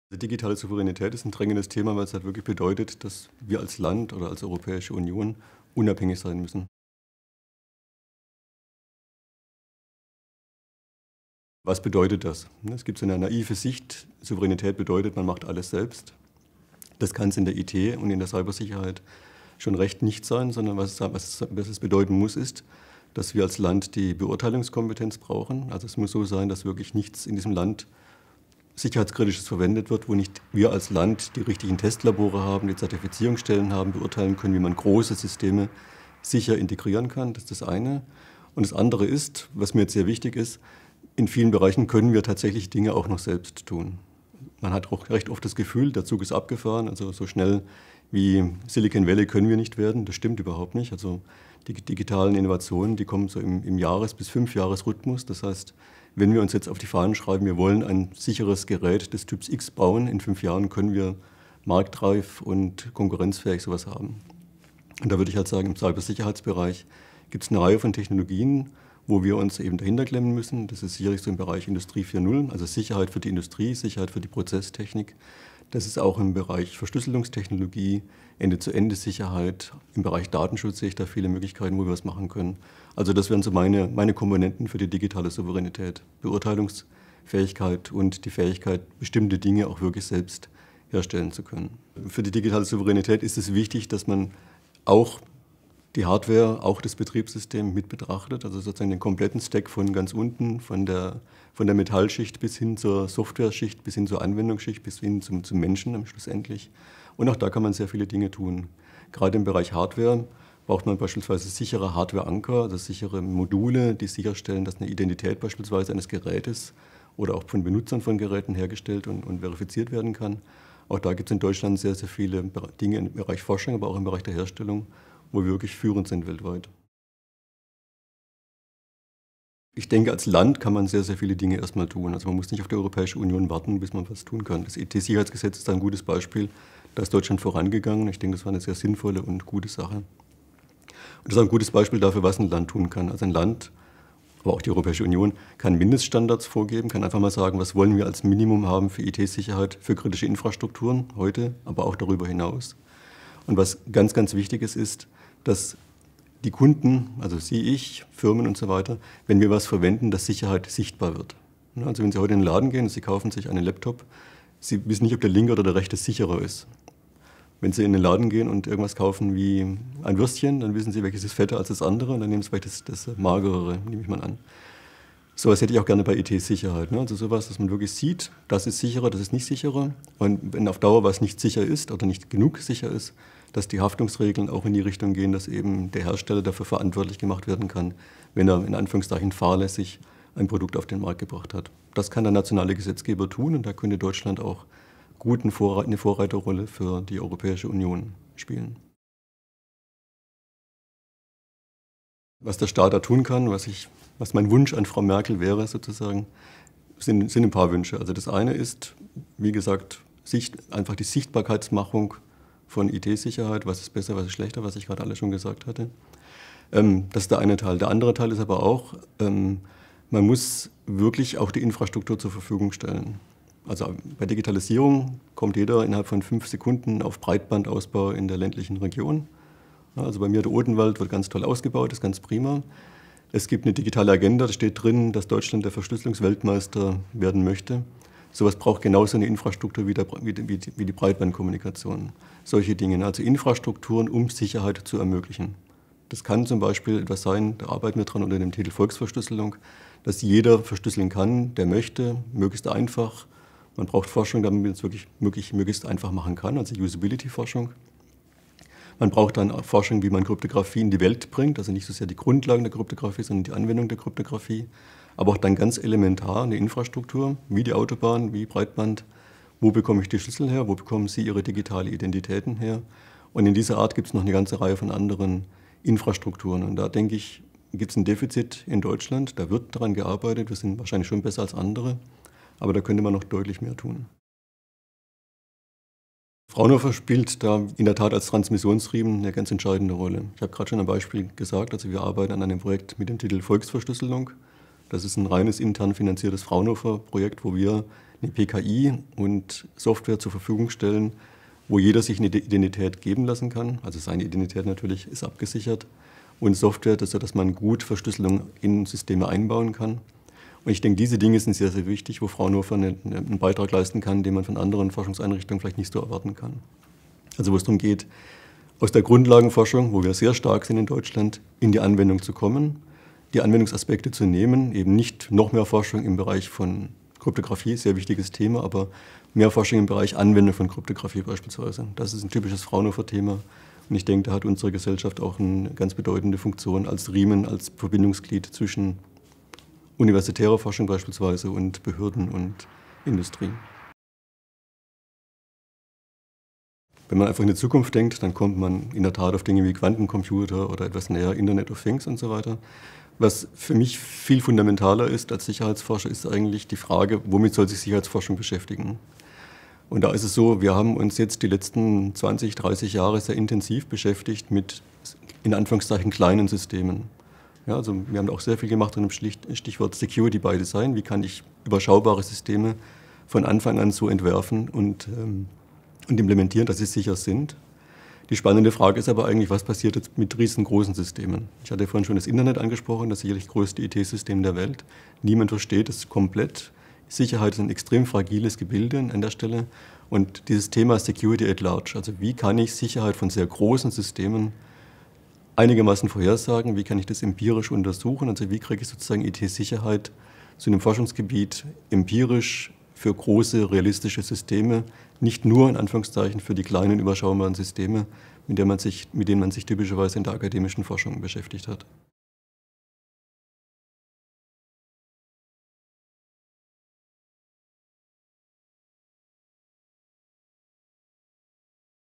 Michael Waidner: Deutschland auf dem Weg zur digitalen Souveränität
Wo steht Deutschland im internationalen Vergleich beim Thema Cybersicherheit? Die hiesige IT-Branche müsse sich gar nicht verstecken, meint Michael Waidner, Leiter des Fraunhofer-Instituts für Sichere Informationstechnologie SIT. Aber vor allem in einem Forschungsbereich sieht er noch akuten Nachholbedarf.

Produktion: Markus Müller, Christian Slezak